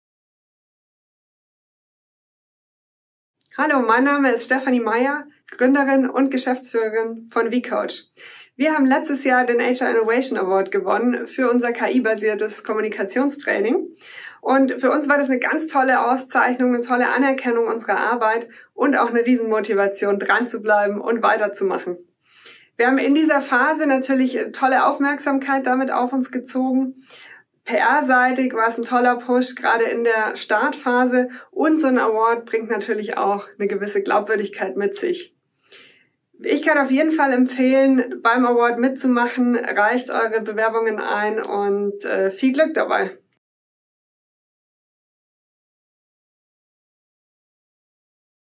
HR Innovation Award: Gewinner vCoach Statement
Ein Jahr nach dem Gewinn des HR Innovation Awards in der Kategorie StartUp gibt uns Stephanie Mayer von vCoach ein kleines Statement was der Award für Sie verändert hat.

Lasst euch inspirieren und sendet uns noch eure Innovationen bis zum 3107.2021. Weitere Informationen findet ihr